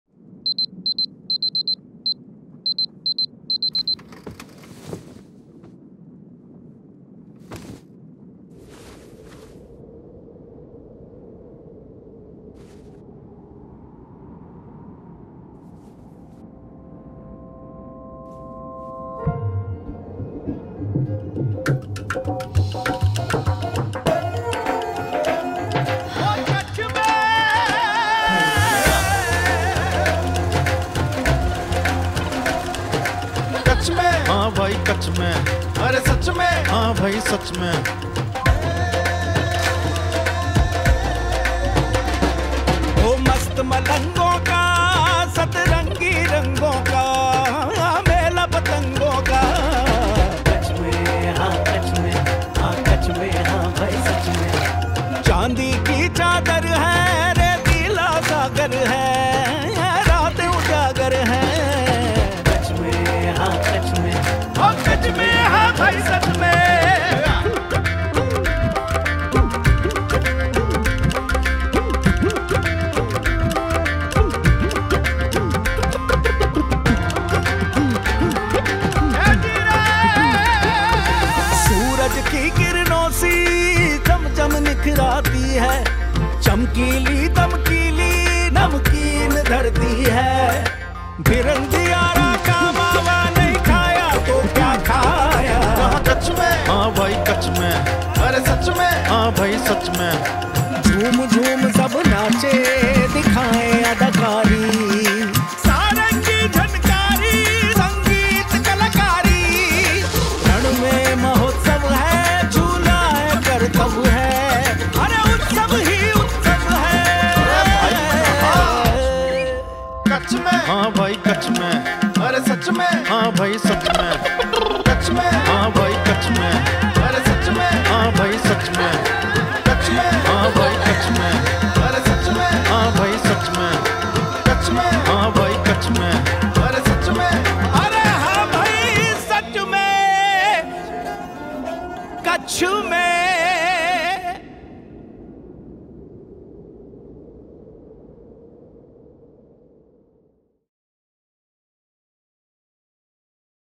Kutch Mein, Sach Mein - Gujarat Tourism
Embark on a mesmerizing journey through the enchanting landscapes of Kutch, from the vibrant cultural hub of Dhordo, the Best Tourism Village, to the celestial beauty of the 'Road to Heaven.' Explore the surreal expanse of the White Rann and immerse yourself in the festive spirit of Rann Utsav. Discover the soul-stirring magic that defines Kutch. Plan your visit today!